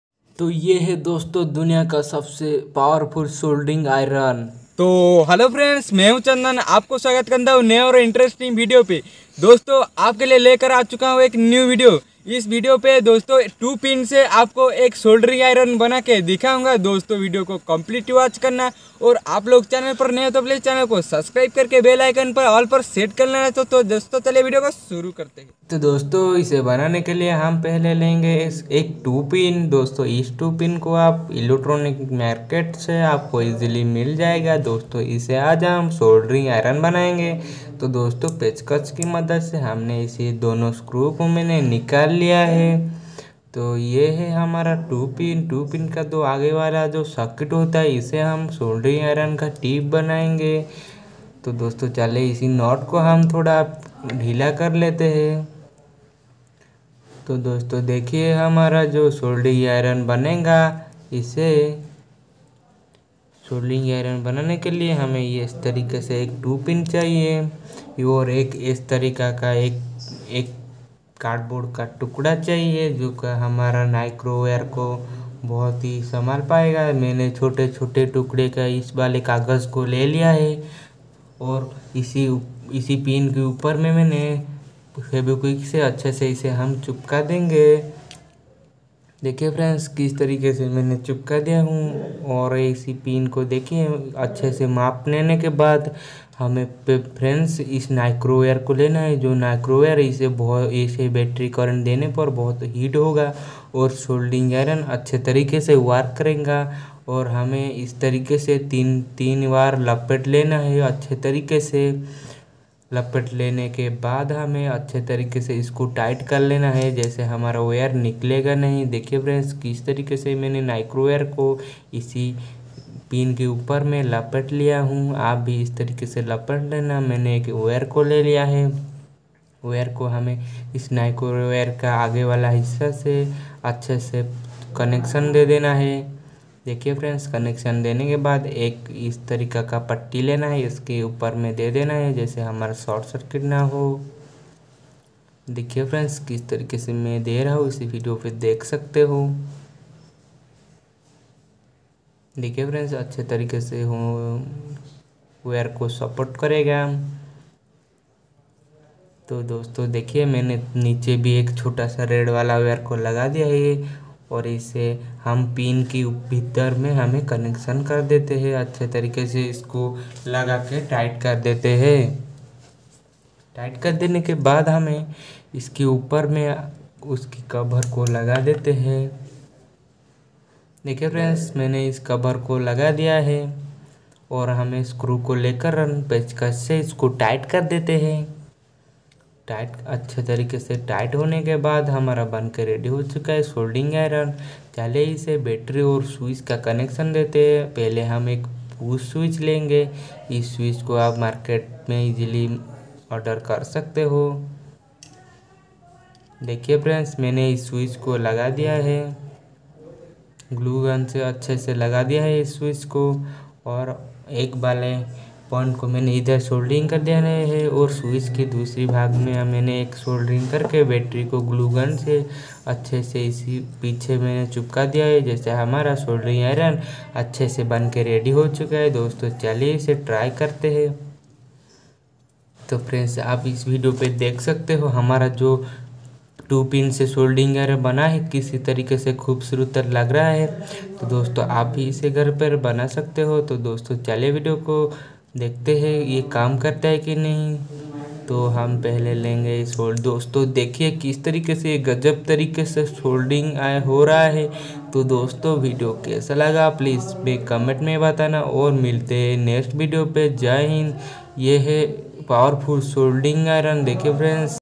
Two pin se solding iron kese banaya || how to make a solding Iron || Chandan Experiment
Aaj aapke liye main Two pin se ek solding iron kese  banaya hai